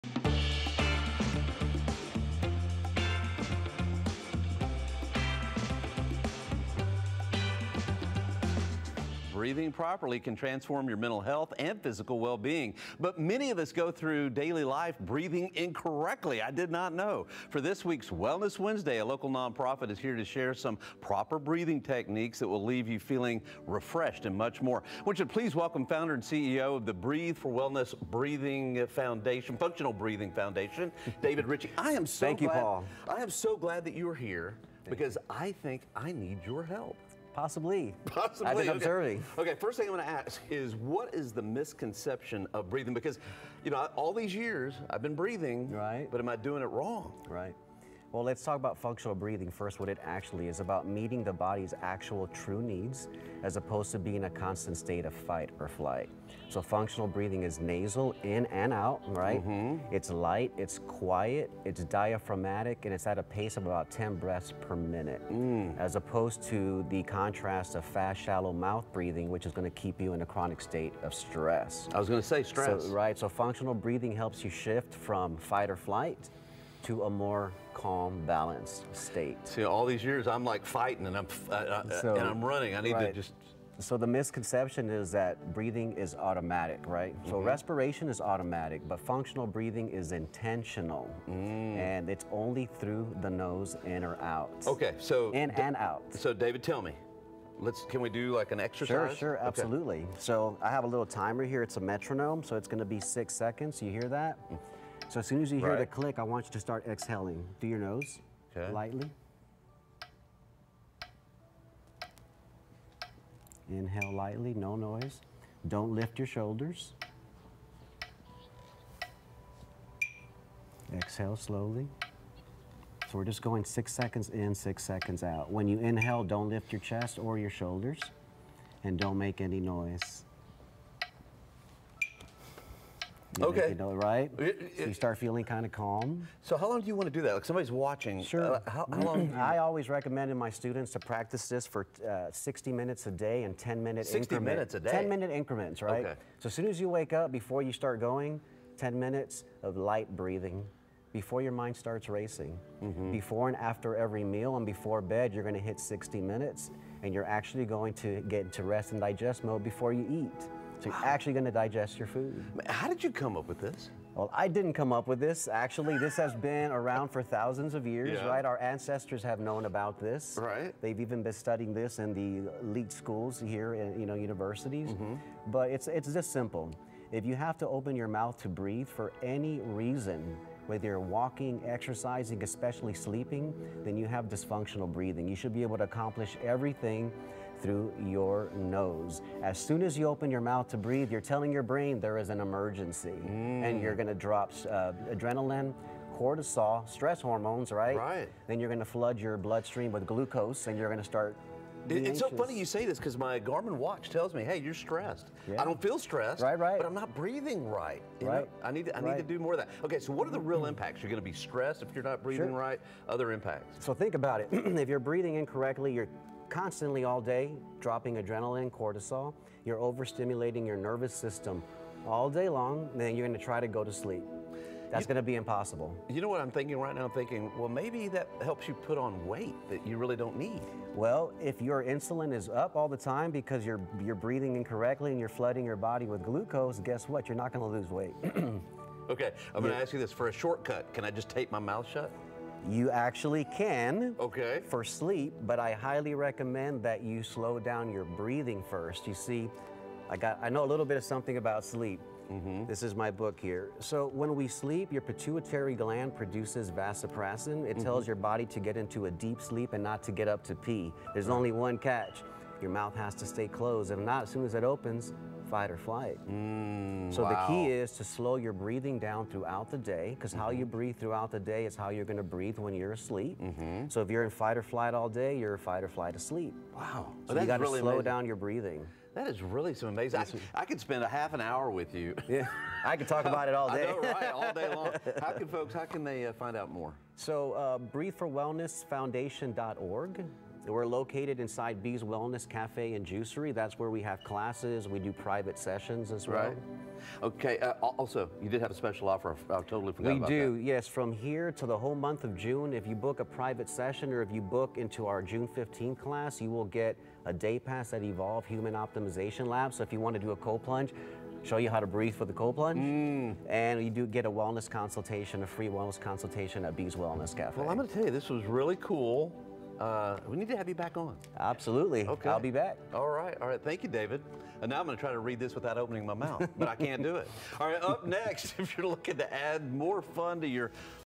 Mastering your breathing | Great Day SA
David Richie with the Breathe for Wellness Foundation teaches Paul functional nasal breathing techniques.